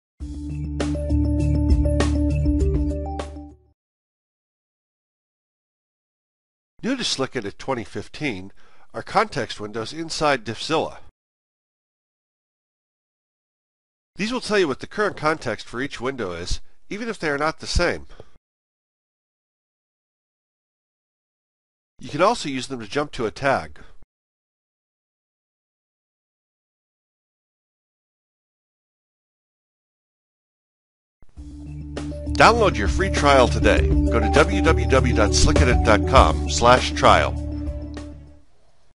SlickEdit - Current Context Windows in DIFFzilla
Demonstration of DIFFzilla with Current Context Windows.

Before you know where you are going, you have to know where you are. Even though displays have gotten bigger and bigger, sometimes you cannot tell what function you are currently working in.

For years now, SlickEdit has had a curent context window docked at the top of the editor to help with this. Now this same convenience has come to DIFFzilla.

Only SlickEdit can offer this feature because it uses SlickEdit's powerful Context Tagging engine.